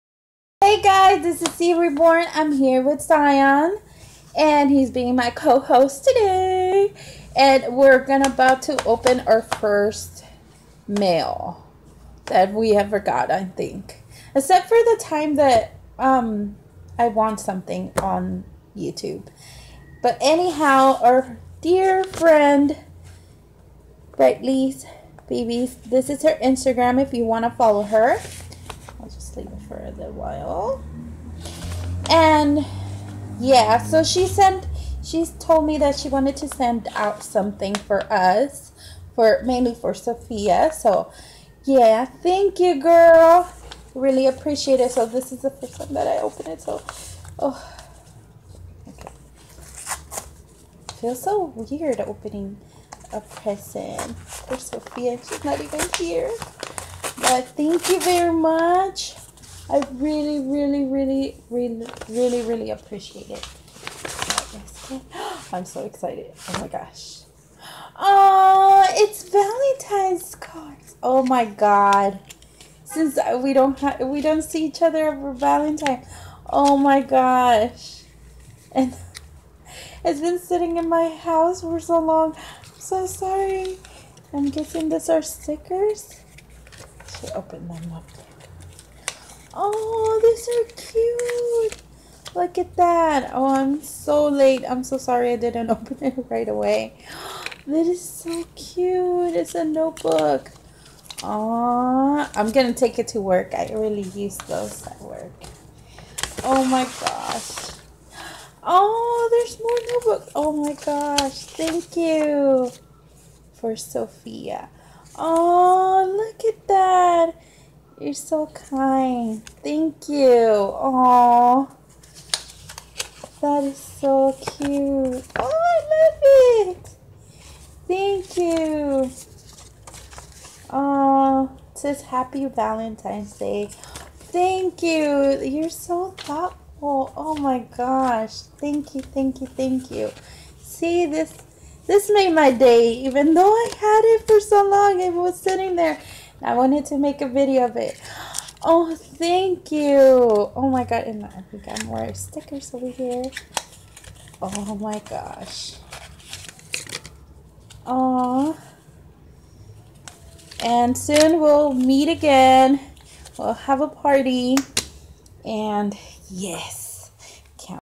Zee-Mail💌 from Brightly.babies
Zion helps me open some awesome ZeeMail from our lovely friend Brightly ❤💚💜💛💌